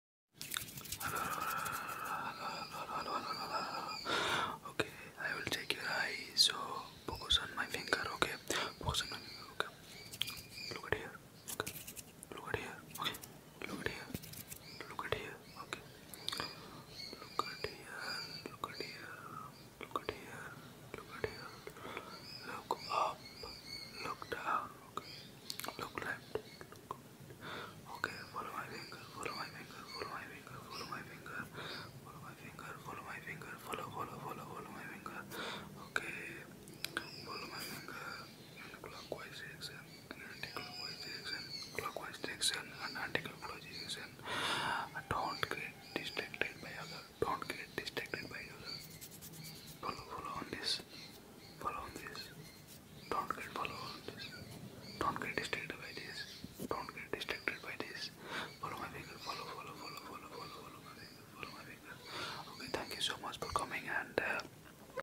ASMR Follow & Focus | Follow My Finger & Attention(1 Minute )
ASMR follow on me ,focus on finger ,attention & personal attention, focus on light , light exam ,eye test examination, a true asmr focus on me for deep sleep video for tingle immunity & for sleep .

i hope ypu will get the all tingles